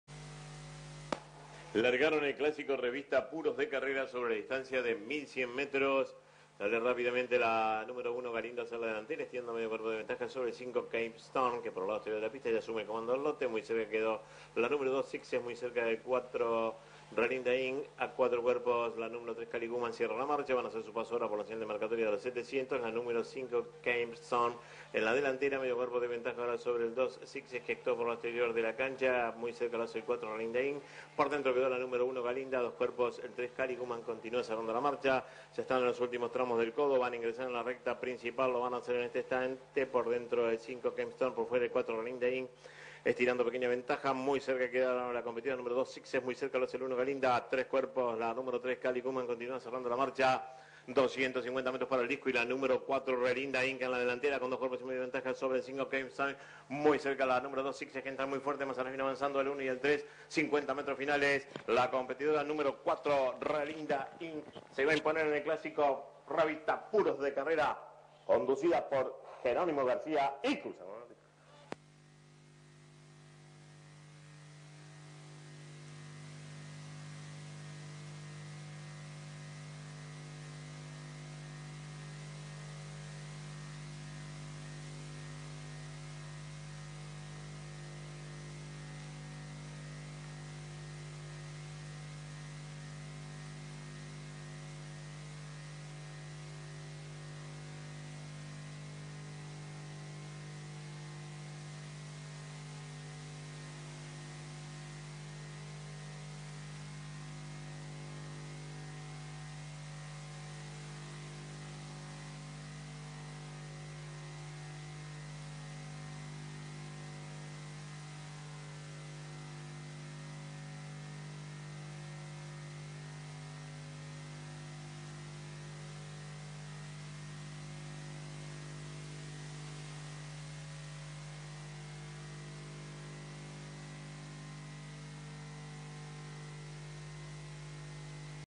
La Plata 041214 5
1 4
Relinda Inc (459)
García, GJ (55.4)
1:03.61 6,60 FS ha
López, I
La Biznaga
2 2
Sixes (488)
Villagra, JC (59)
5 cps 2,85 FS aa
Mondazzi, EA"
Elsie
3 5
Cape Storm (455)
Gonçalves Fernándes, FL (59)
1 cp 4,25 FS aa
Mayansky Neer, JA"
La Manija
4 1
Galinda (448)
Alderete, OA (59)
1 cp 5,75 FS aa
Sueldo, HA
Abuela Chola (LP)
5 3
Kali Woman (463)
Menéndez, CD (59)
hco * 2,45 -- aa
San Millán, IL"
Stud-Jo